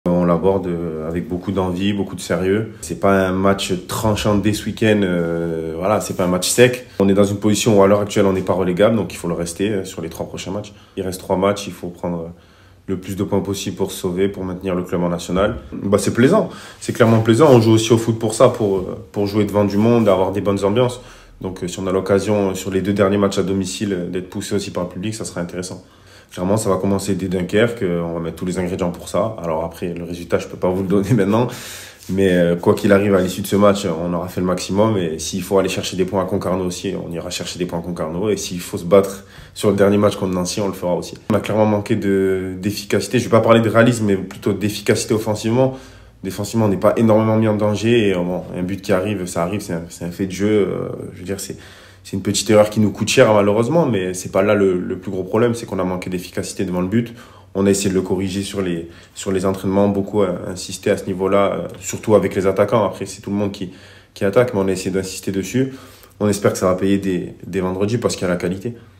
Avant-match J32 : Leandro Morante
À quelques heures de la réception de Dunkerque dans un stade Verchère rempli, Leandro Morante était en conférence de presse. Il a notamment évoqué la fin de saison et l'approche qu'a le groupe sur ce sprint final.